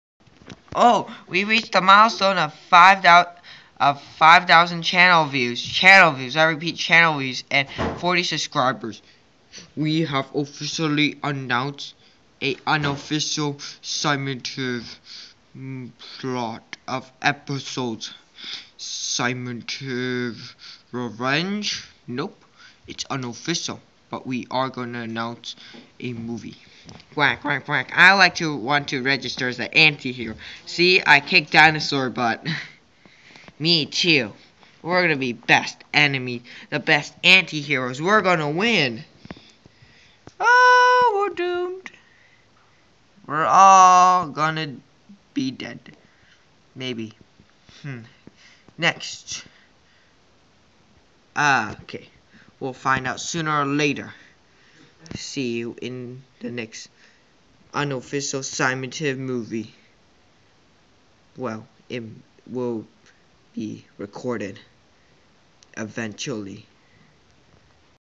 Simontv17 40 Subcribers & 5000 Channel Views Special!
Wow! What a milestone! Keep on smashing those likes.

Duckhol announces a new series of plotted Simontive like a Tv series.

Support simontv17 by passing out flyers.